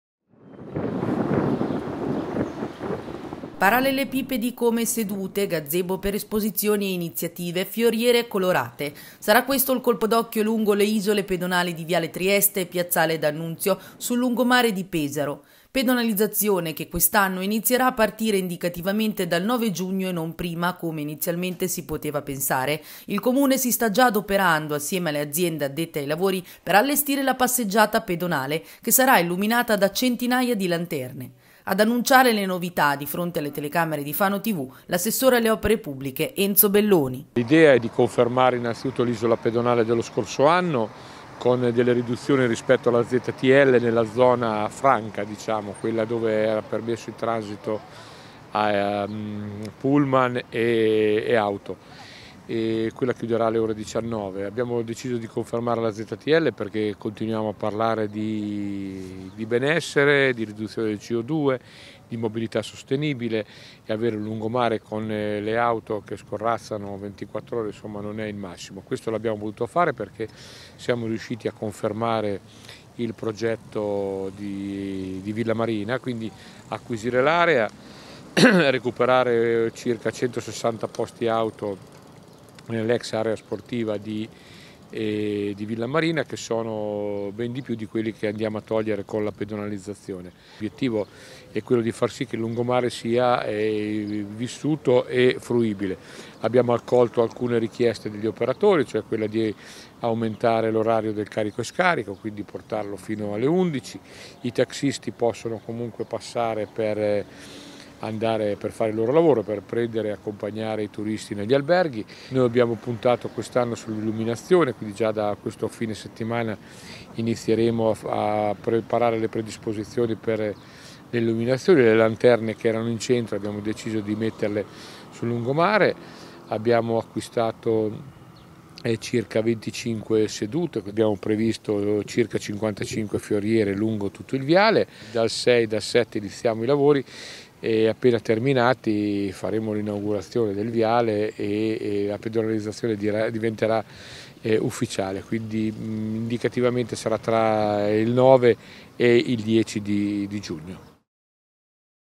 Al via le isole pedonali sul lungomare di Pesaro
Il lungomare di Pesaro si rifà il look. Lanterne luminose, fioriere colorate, sedute moderne lungo Viale Trieste. Il Comune è al lavoro per realizzare le nuove isole pedonali.